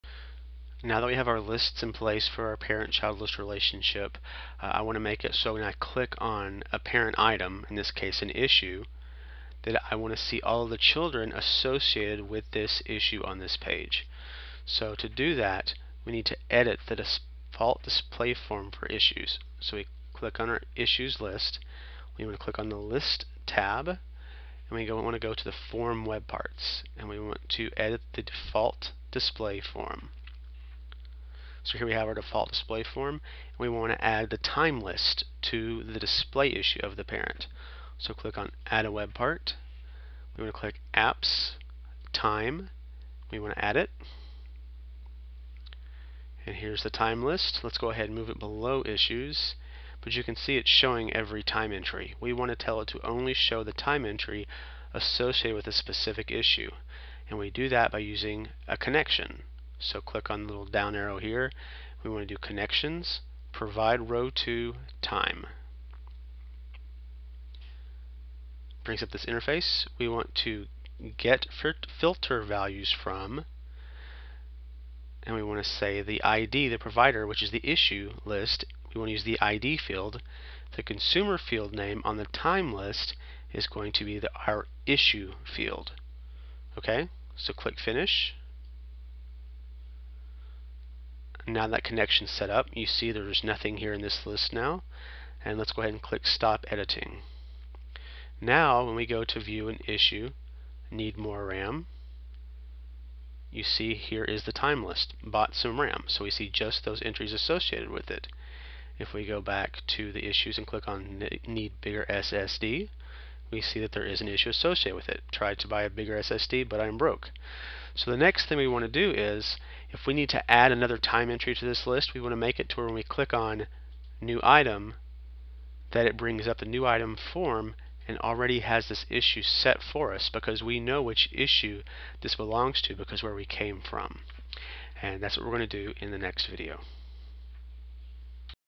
SP2013 Parent/Child List - Create Web Part Connection
Demonstrates how to add a child list to the display form of a parent liste item and set up a web part connection between the two.